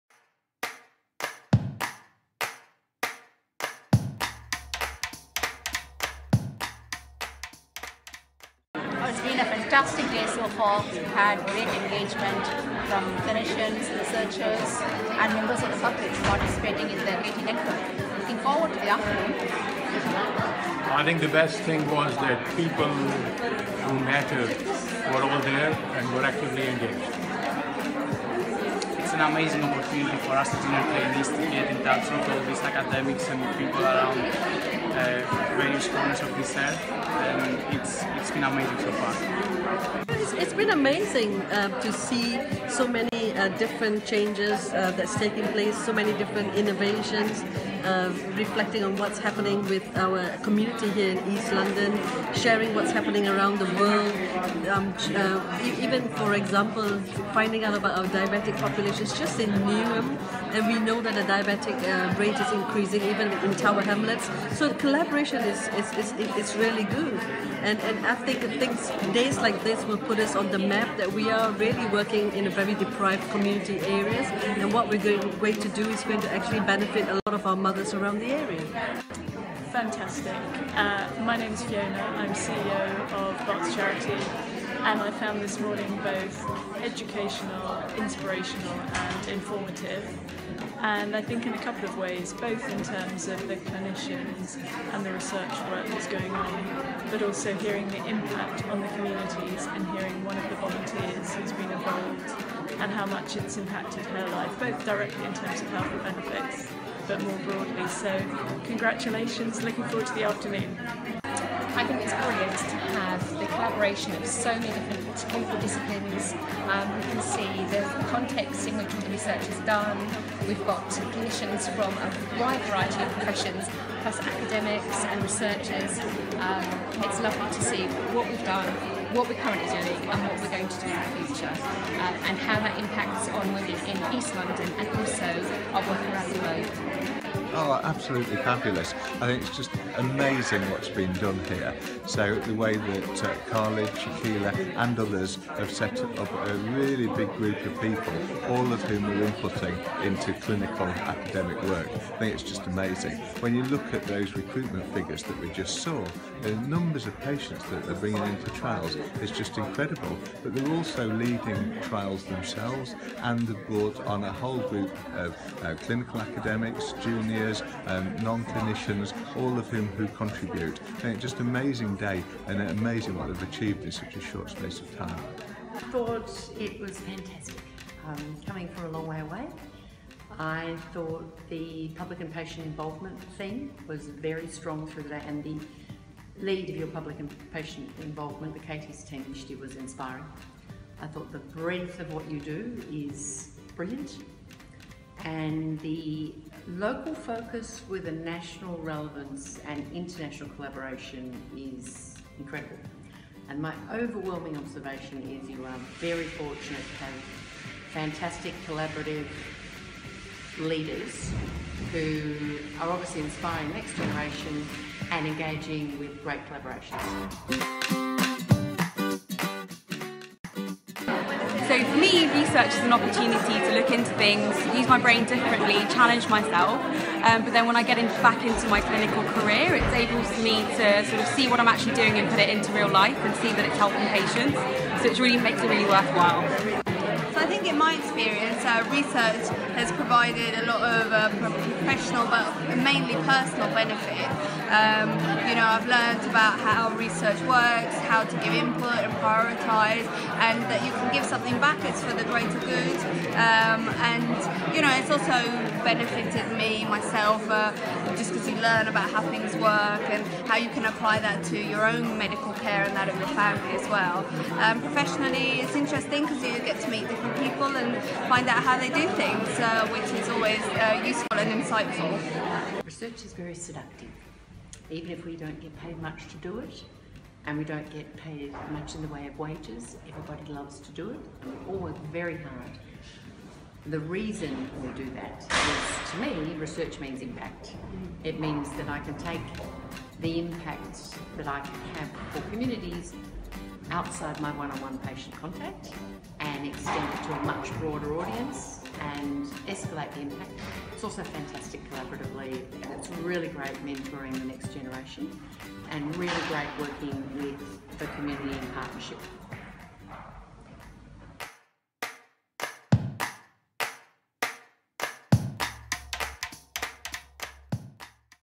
KT Network 6th annual meeting: 10th June 2016
Highlights from the KT Network 6th annual meeting on the 10th June 2016.